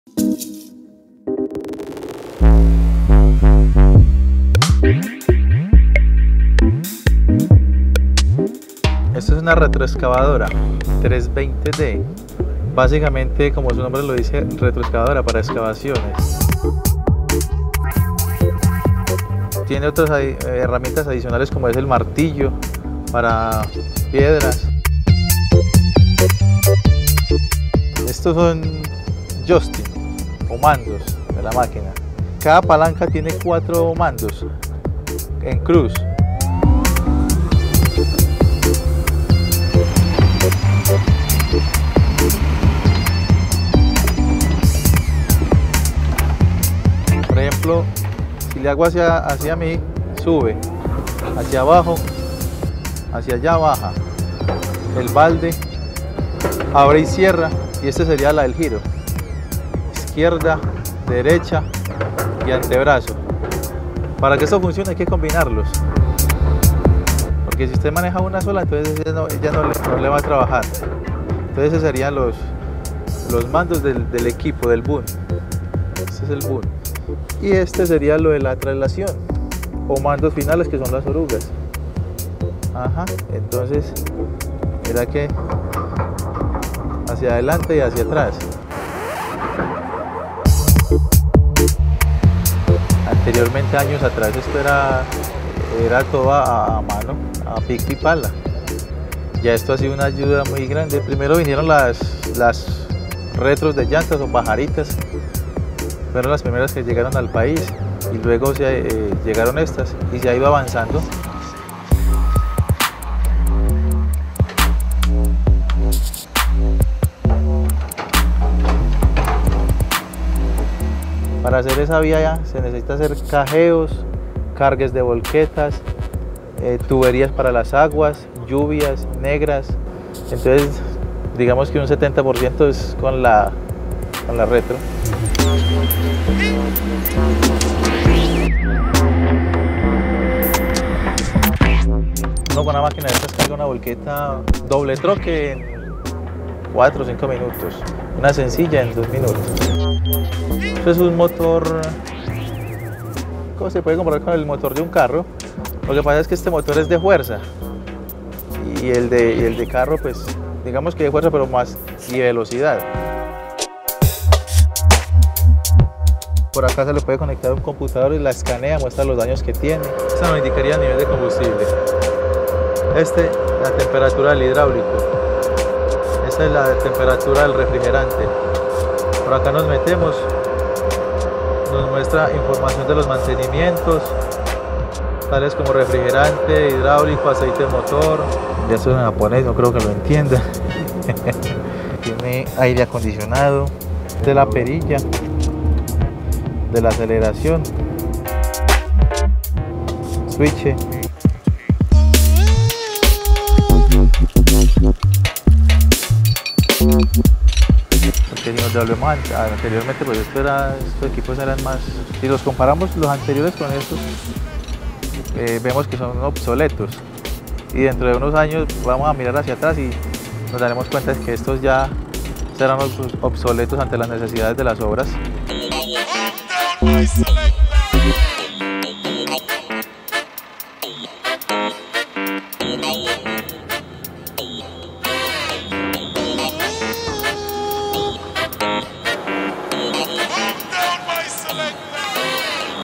Retroexcavadoras
Desde la invención de las retroexcavadoras, el ser humano vio que su trabajo en la construcción de grandes obras, se ha visto reducido en mucho y gracias a ellas, se ha hecho cada vez más rápido.

 Nosotros, estuvimos en una obra, hablando con el operador de una máquina de esas que le ayudan a diario al hombre para construir y conocimos cuáles han sido esos grandes avances que ha representado para el mundo, cómo se manejaba y cuánto trabajaba. Veamos.